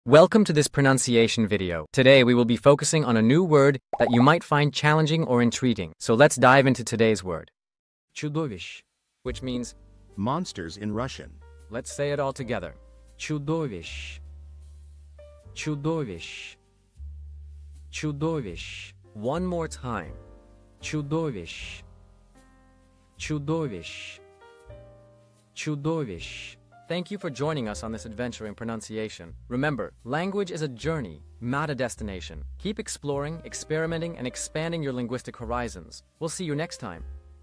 How to pronounce Chudovisch | Чудовищ (Monsters in Russian)
Master the Pronunciation of 'Chudovisch | Чудовищ' - which means : Monsters in Russian 📝 with @PronunciationRussian 🎓🗣️ - [Your Guide to Russian]

Learn to pronounce 'Chudovisch' perfectly in Russian with our easy and engaging tutorial by @PronunciationRussian! 📖📽️ In this comprehensive video, we provide you with native speaker examples 🗣️👂 and phonetic breakdowns 🎵 to ensure you get the pronunciation just right.